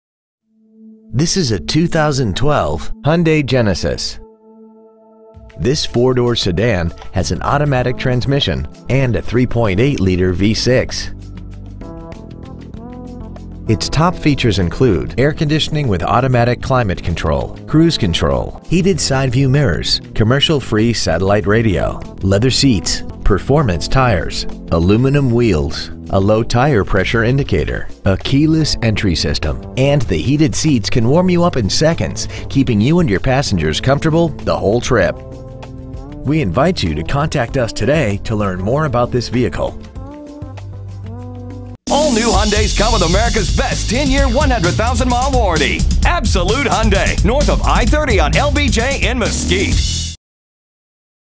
Preowned 2012 Hyundai Genesis TX Garland TX
Year : 2012
 Make : Hyundai
 Model : Genesis
 Engine : 3.8L V6
 Trans . : Automatic
 Exterior : White
 Miles : 15
 Interior : Tan
 Stock : CU165513